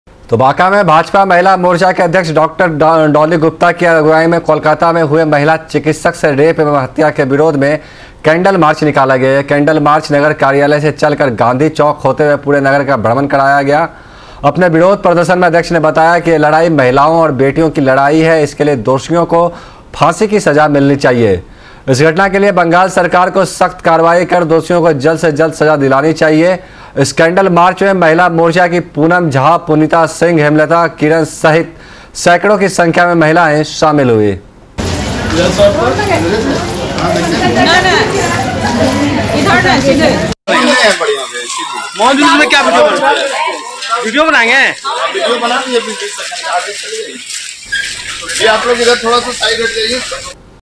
बांका : कोलकाता रेप कांड के विरोध में भाजपा ने निकाला कैंडल मार्च
Raftaar Media